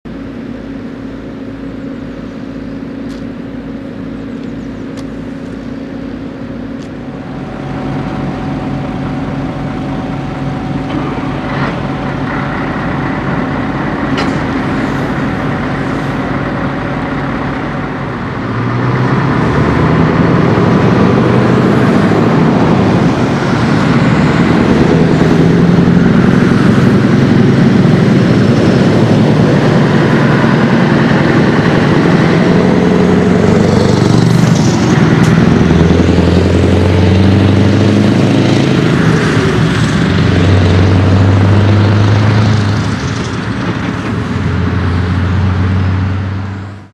scania v8 480 - g.doesburg
G.doesburg opent de pijp en rijdt weg met zn scania 480 v8.